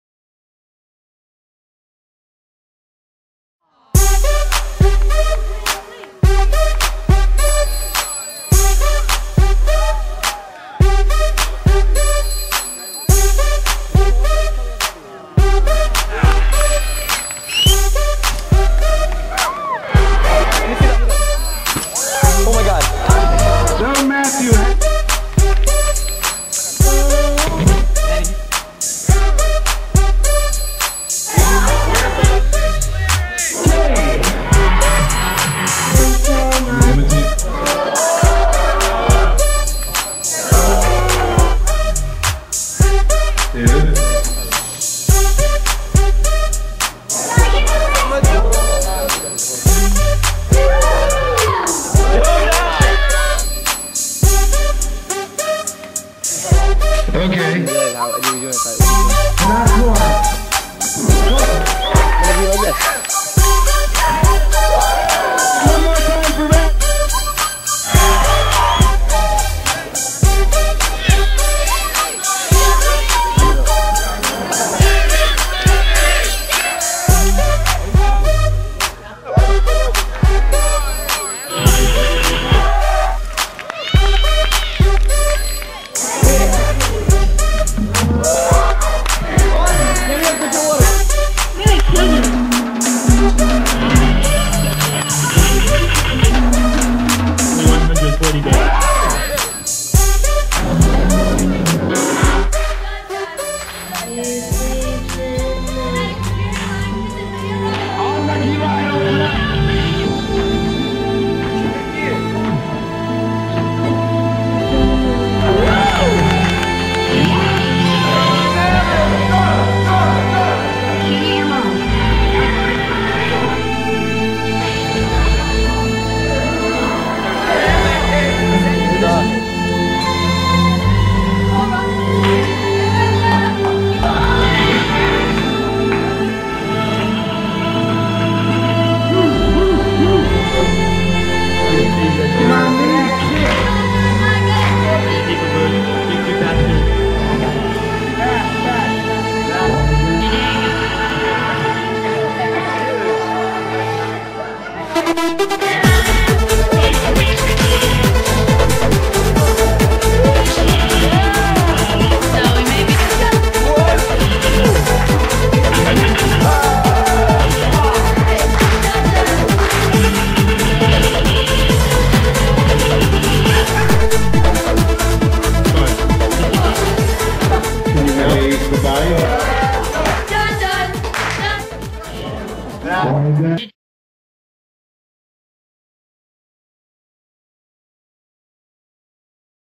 Nageela Idol 06 Remix
Clips from talent show in Camp Nageela (East) summer of '06.
Featuring such world renowned (almost) names as  Dzgeophf Weisz, Goob, and Sam Lipke.